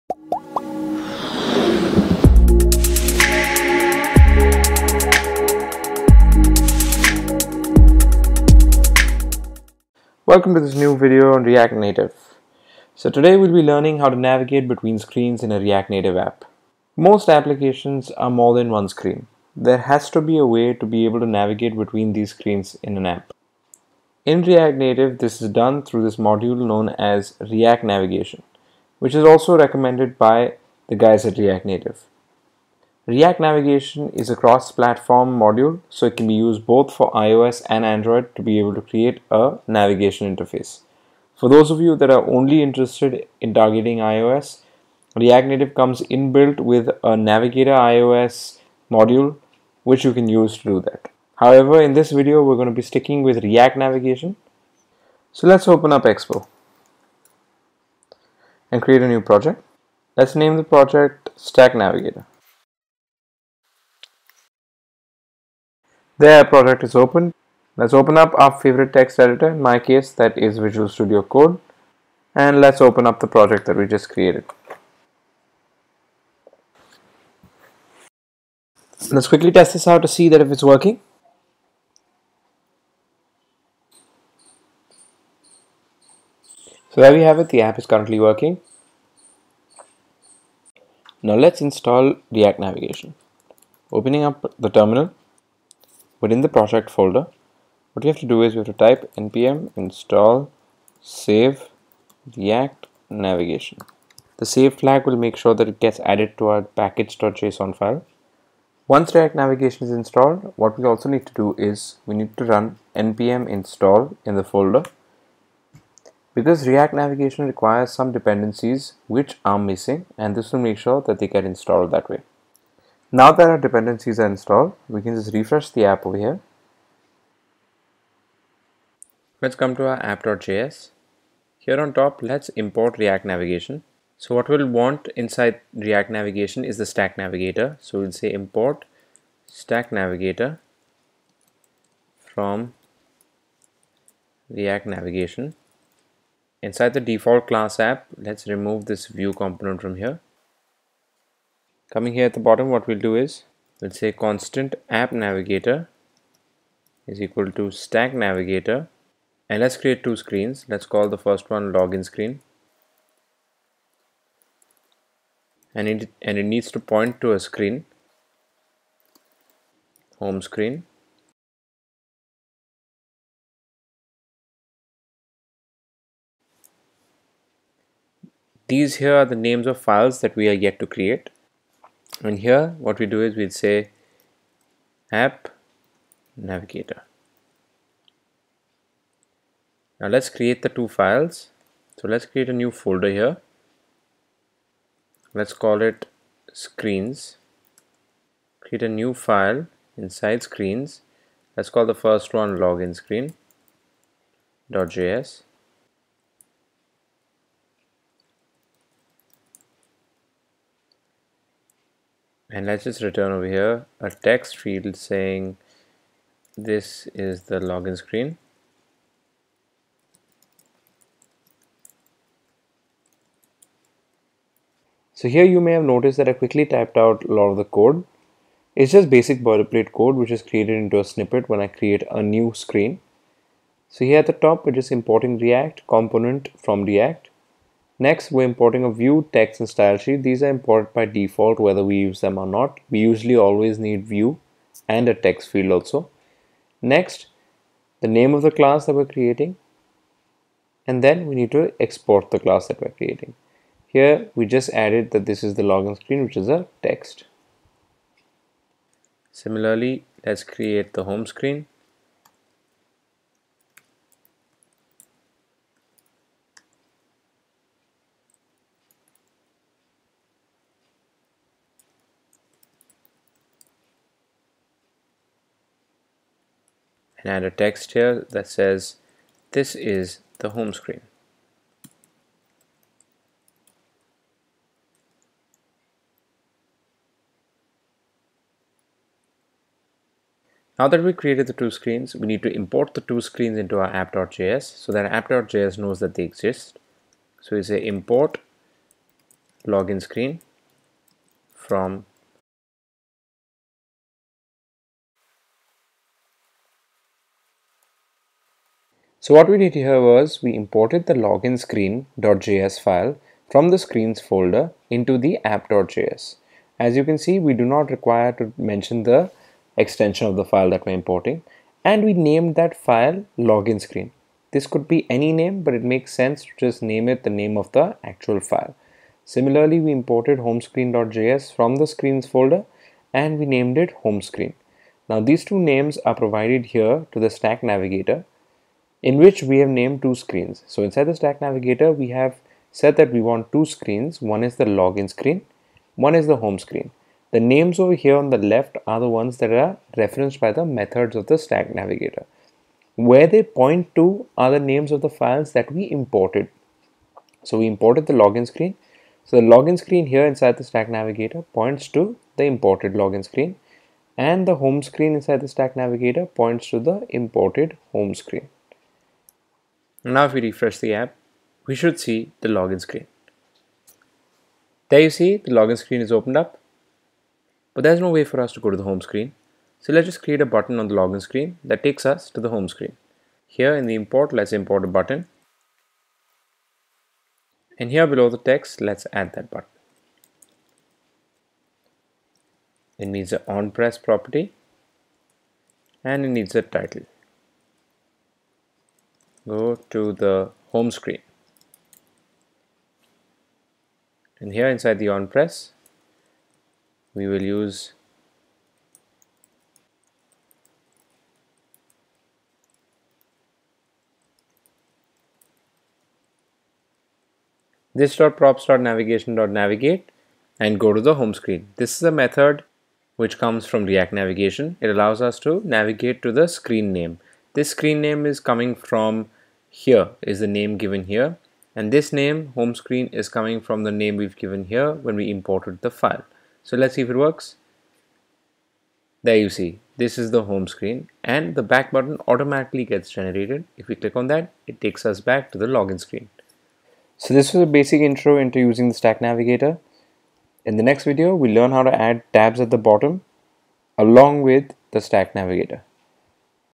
#3 React Native - React Navigation V1 | Stack Navigator | Expo.io
An intro to using the stack navigator in react navigation for your react native app.
Real World Practical Example.

New To React Native?